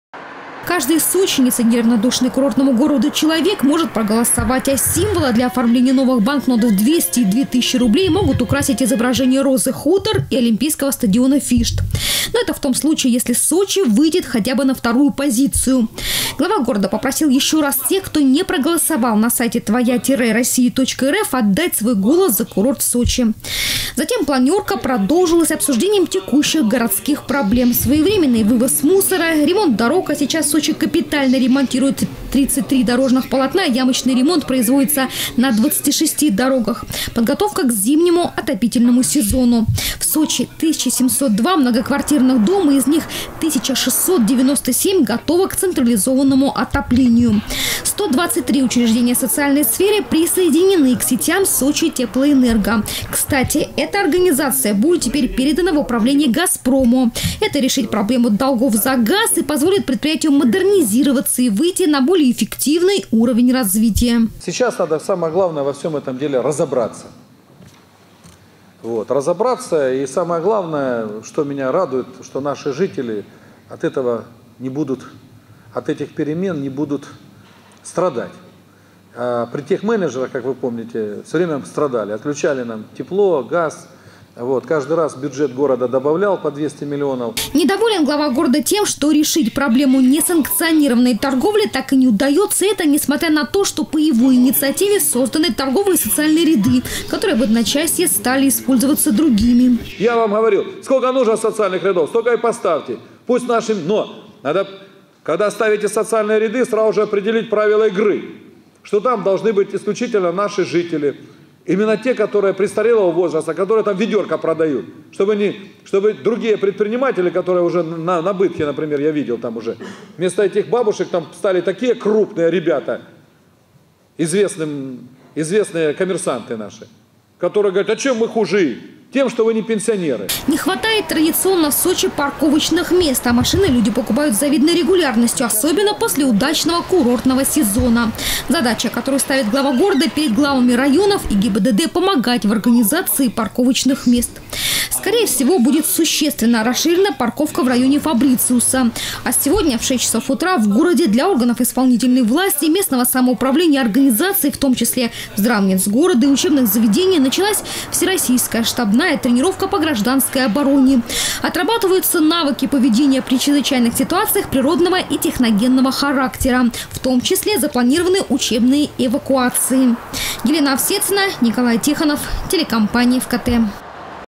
"Сочитеплоэнерго" передадут в управление Газпрому. Новости Сочи Эфкате
Это и другие темы были рассмотрены сегодня во время планерного совещания главы города.
Сочи. Новости.
Телеканал Эфкате.